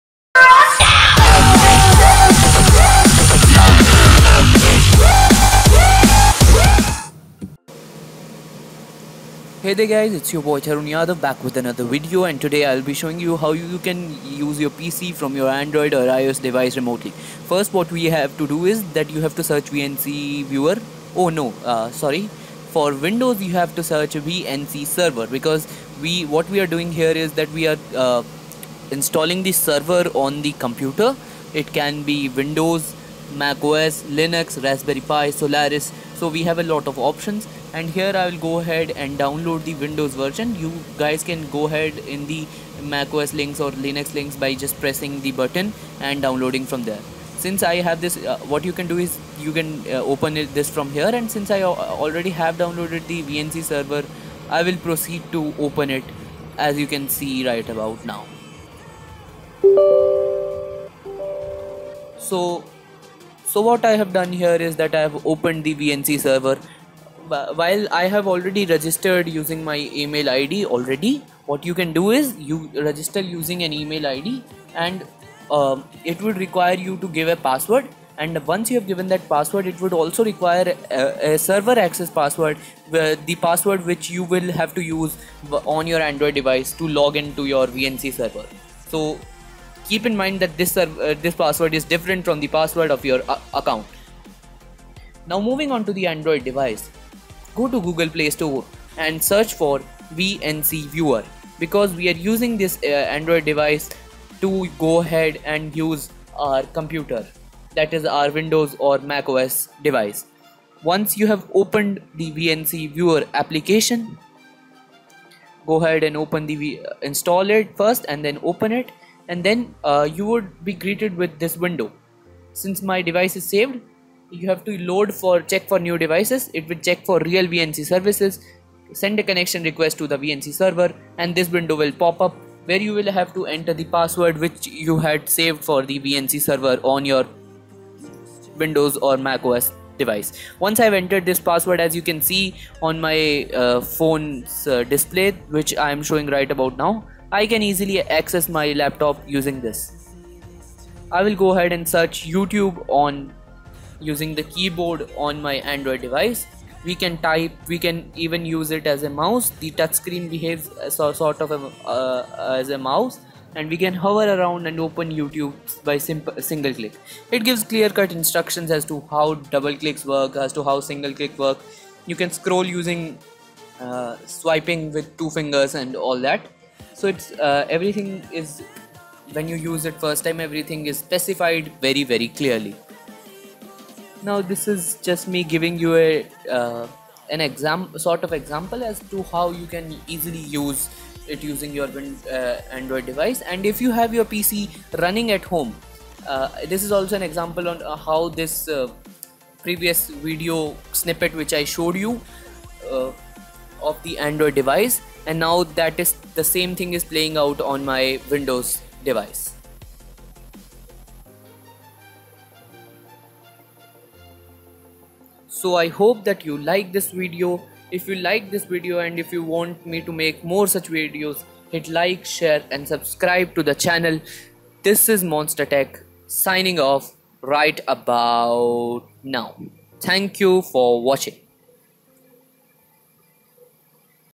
How to control PC with android phone
Hey there guys this is Tarun Yadav from Monster Tech and today I will show you how by using VNC Server and VNC Viewer application you guys can easily access you Windows or MacOS Laptop/ PC from an android or ios device like android phone/tablet or even ipod or iPad from Apple! Just watch this small video to get an idea how you can do this very easily!